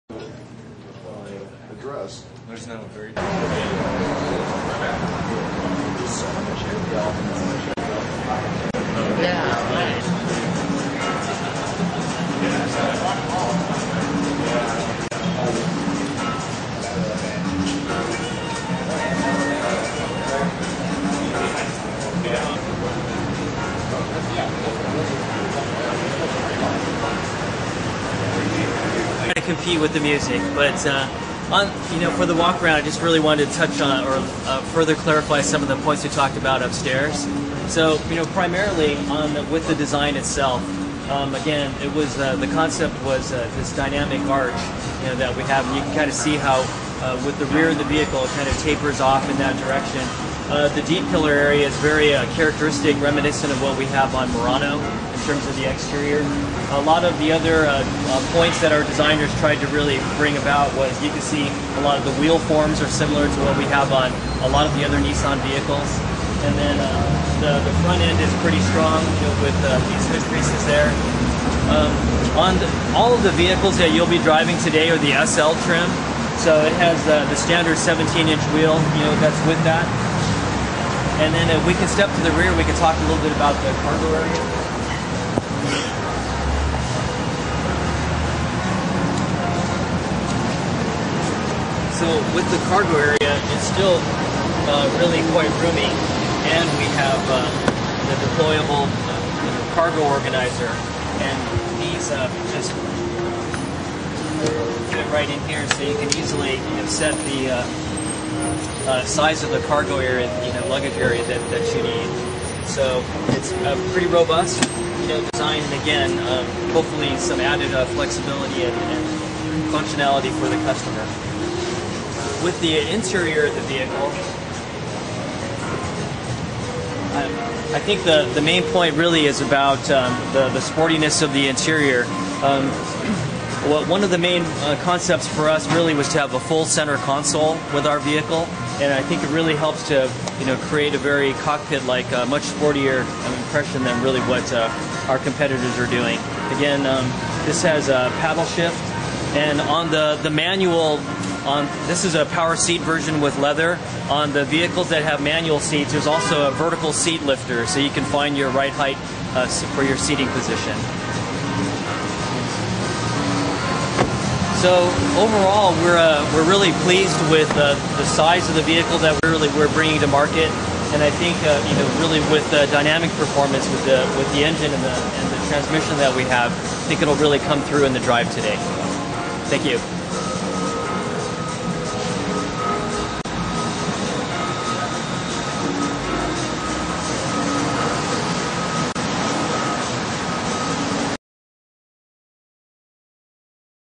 2008 Nissan Rogue, Walk Around
for the largest collection of new car introductions.
Take a walk around the new 2008 Nissan Rogue with Ken Kcomt, manager.